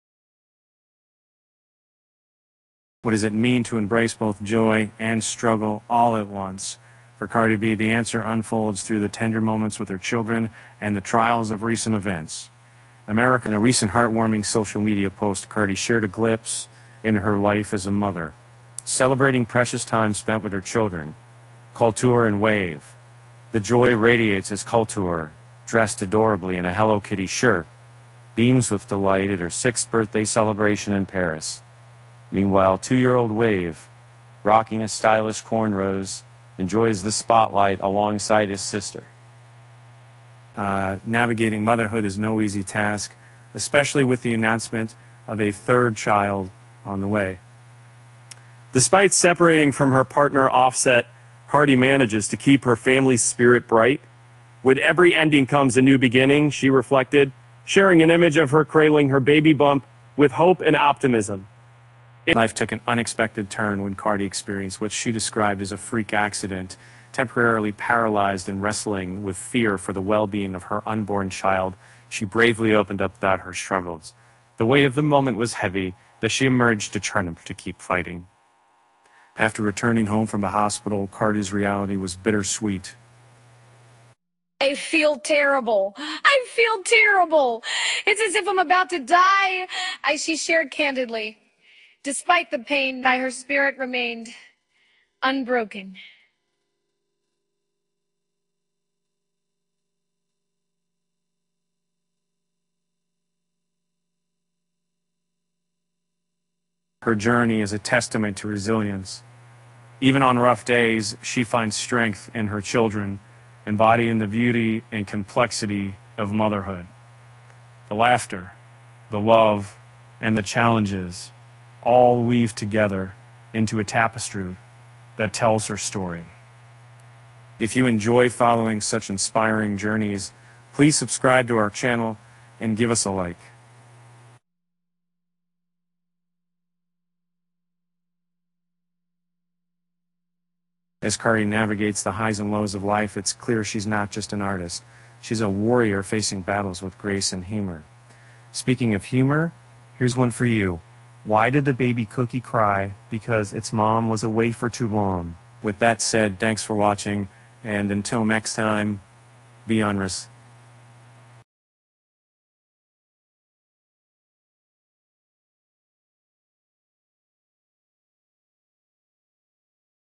Pregnant Cardi B shares sweet video with her kids Kulture, six, and Wave, two, after suffering a 'fr
What does it mean to embrace both joy and struggle all at once? For Cardi B, the answer unfolds through the tender moments with her children and the trials of recent events.